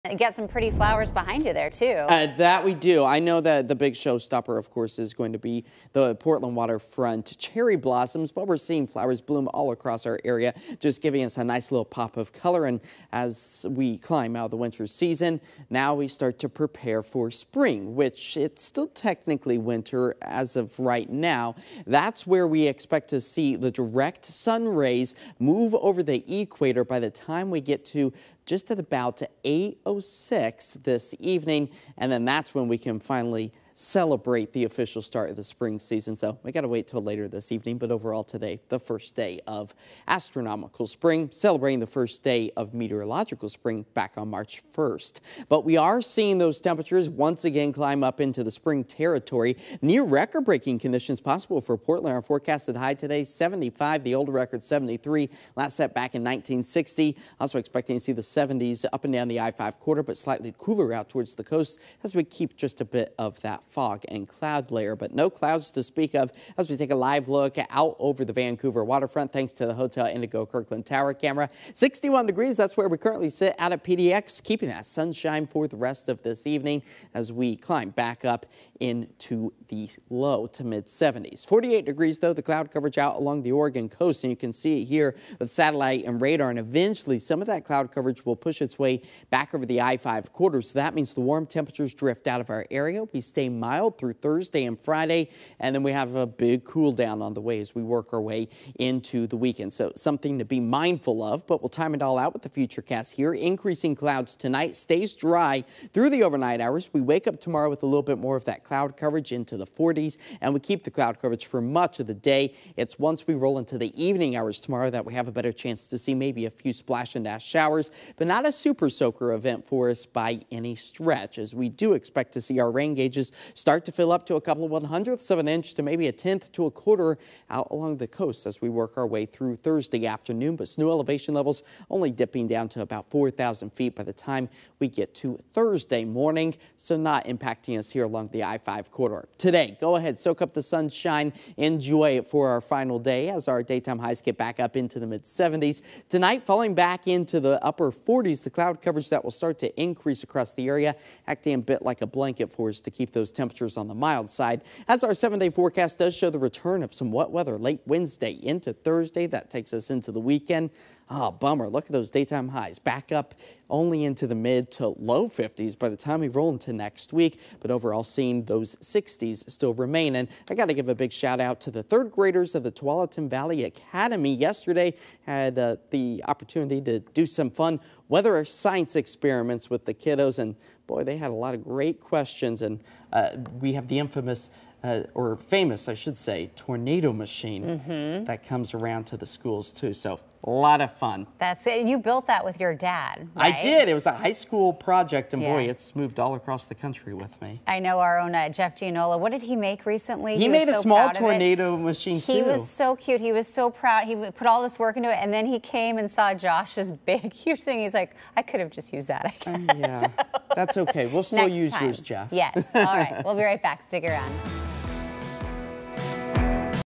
Another round of record warmth possible in Portland Tuesday
KOIN 6 Meteorologist Josh Cozart shares Portland's potential to see another round of record warmth Tuesday afternoon.